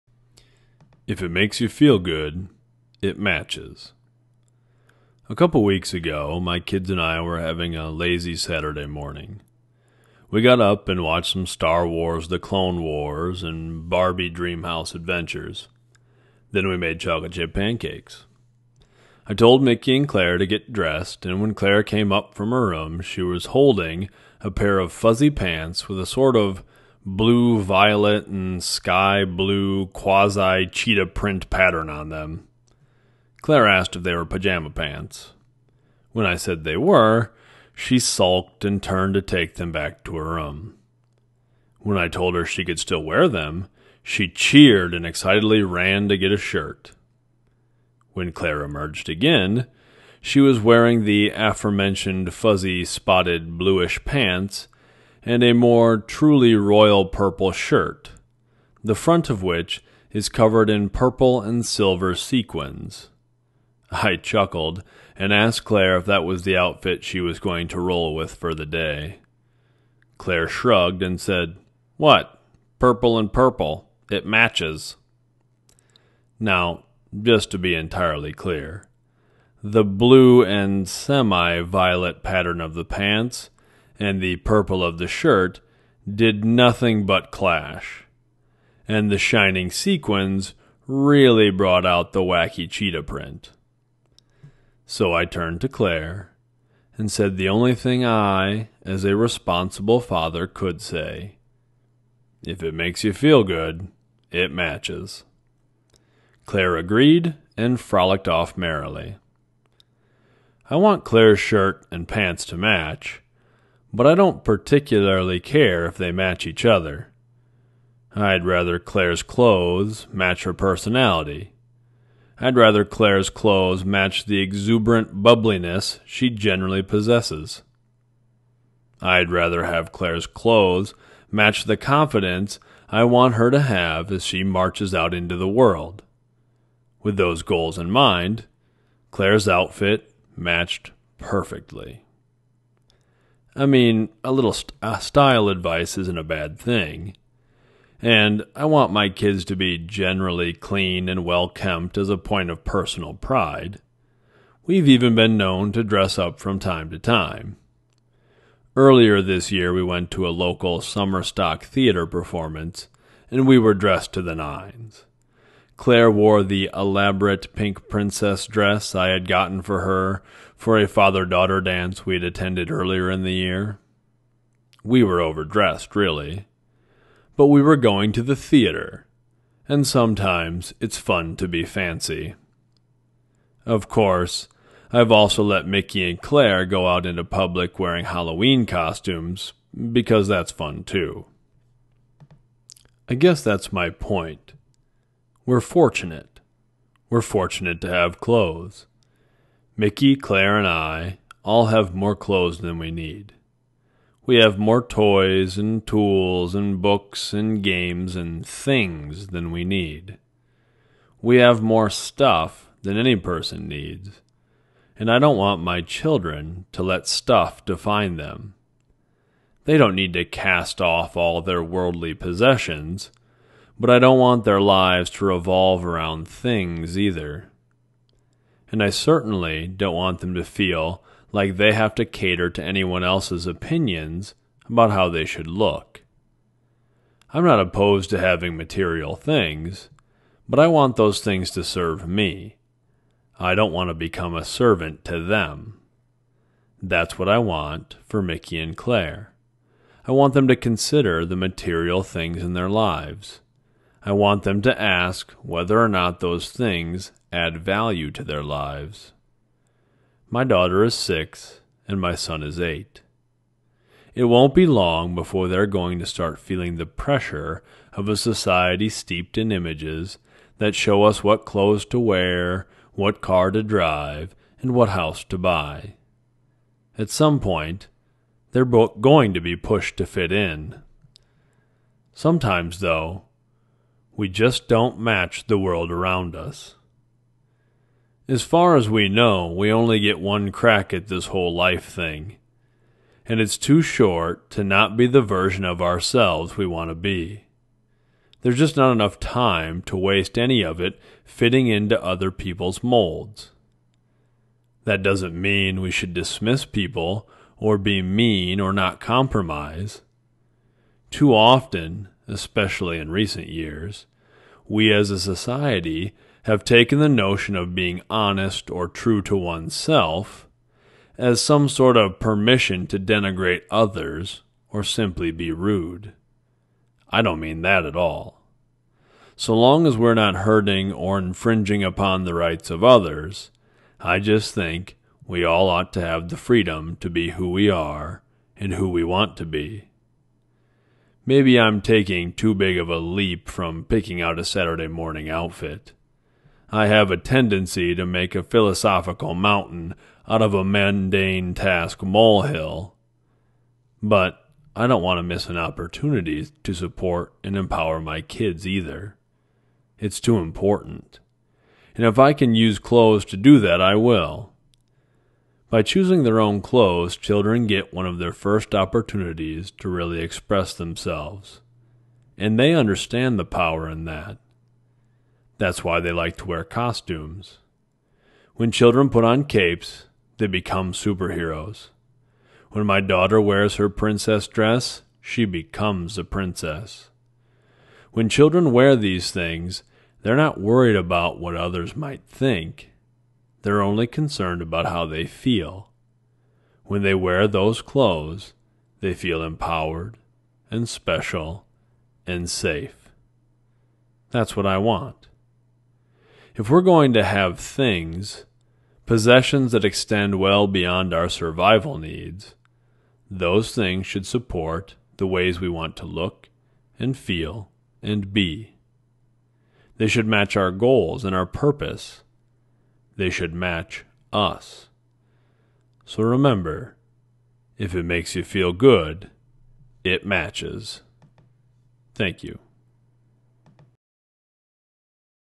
Audio Companion: "If It Makes You Feel Good, It Matches" (10/28/18)
I discuss confidence, being yourself, and a pretty interesting outfit my daughter picked out for herself.